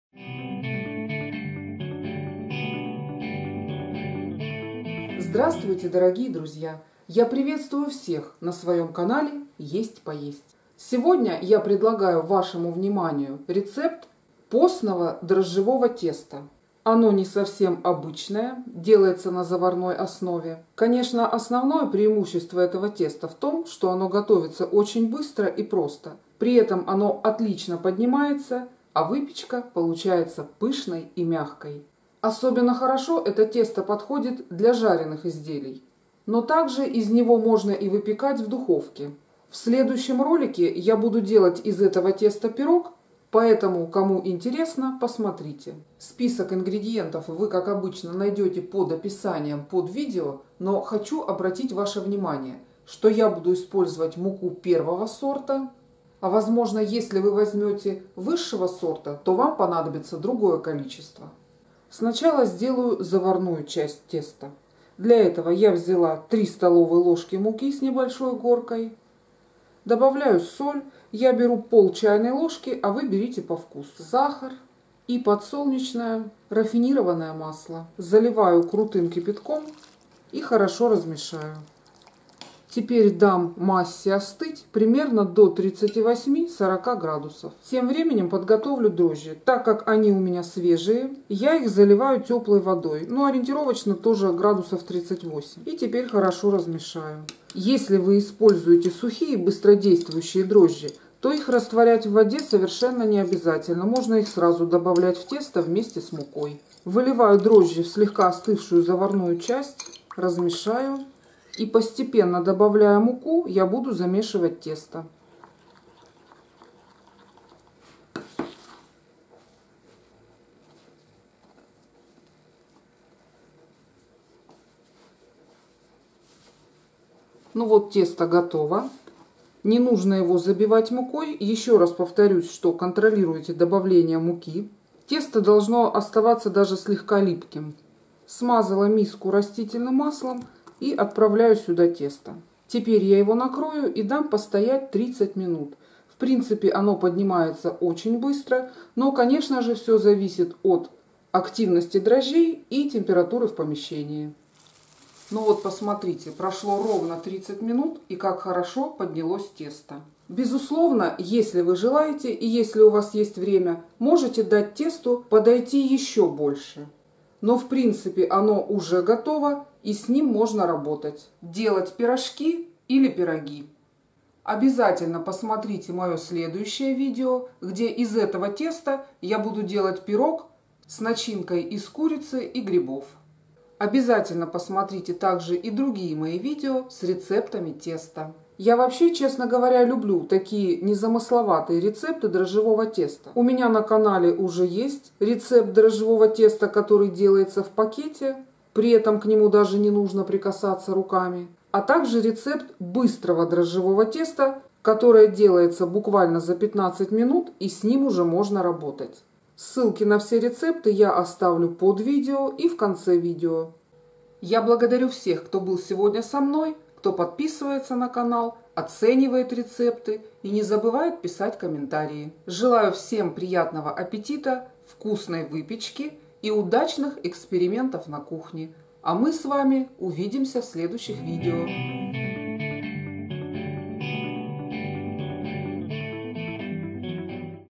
Постное заварное дрожжевое тесто - пышное и мягкое
Постное заварное дрожжевое тесто готовится очень быстро, ему не нужно много времени для расстойки. При этом выпечка хорошо поднимается и получается пышной и мягкой, будто сдобная. Из него можно как печь в духовке, так и жарить в масле. Начинка подойдёт любая, не обязательно постная.
Ингредиенты
заварная часть:
150 мл кипятка,
3 ст.л муки,
3 ст.л растительного масла,
1 ст.л сахара (или по вкусу),
1/2 ч.л соли( или по вкусу)
основная часть:
150 мл тёплой воды,
400 г муки (ориентировочно) я беру 1 сорта,
5 г сухих дрожжей или 15 г свежих.